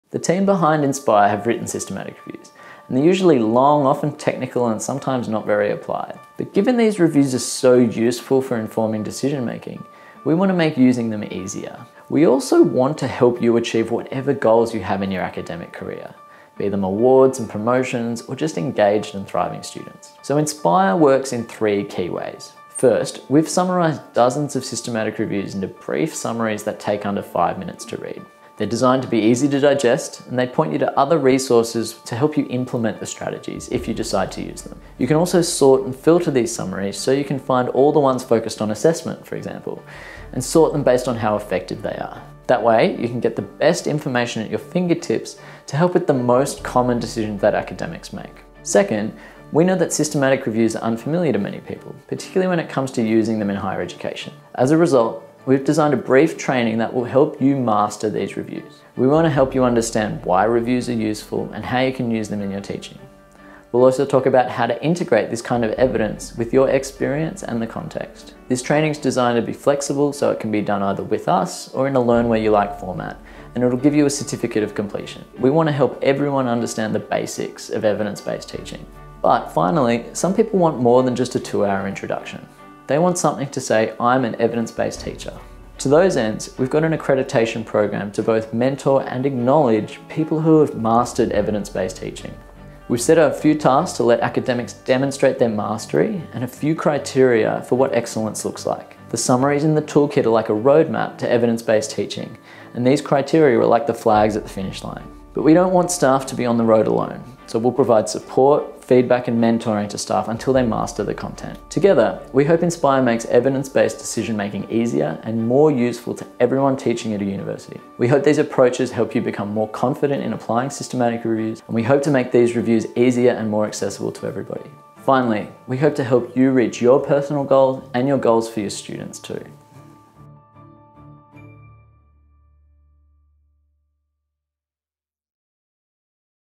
How INSPIRE is built to help you: resources built for busy academics
At INSPIRE, we’ve written systematic reviews and they’re usually long, often technical, and sometimes not very applied. But, given these reviews are so useful for informing decision-making, we want to make using them easier. We also want to help you achieve whatever goals you have in your academic career, be them awards and promotions, or just engaged and thriving students.
So, INSPIRE works in three key ways. First, we’ve summarised dozens of systematic reviews into brief summaries that take under 5 minutes to read. They’re designed to be easy to digest, and they point you to other resources so you can implement the strategies, if you decide to use them. You can also sort and filter these summaries, so you can find all the ones focused on assessment, for example, and sort them based on how effective they are. That way, you can get the best information at your fingertips to help with the most common decisions that academics make.

Second, we know systematic reviews are unfamiliar to many people, particularly when it comes to higher education. As a result, we’ve designed a brief training program that will help you master these reviews. We want to help you understand why reviews are useful and how you can use them in your teaching. We’ll also talk about how to integrate this kind of evidence with your expertise and the context. This training is designed to be learn-when-you-like and will give you a certificate of completion. Basically, we want to help everyone understand the basics of evidence-based teaching.
But, finally, some people want more than just a 2-hour introduction. They want something to say “I’m an evidence-based teacher.” To those ends, we’ve got an accreditation program to both mentor and acknowledge people who have mastered evidence-based teaching. We’ve set out a few tasks to let academics demonstrate their mastery and set a few criteria for what excellence looks like. The summaries in the toolkit are like a roadmap to evidence-based teaching, and these criteria are like the flags at the finish line. But, we don’t want staff to be on the road alone, so we’ll provide support, feedback, and mentoring to staff until they master the content.

Together, we hope INSPIRE makes evidence-based decision-making easier and more useful to everyone teaching at university. We hope these approaches help you become more confident in applying systematic reviews. We hope to make these reviews easier and more accessible to everyone. And, we hope to help you reach your personal goal and your goals for your students too. Inspirational, huh?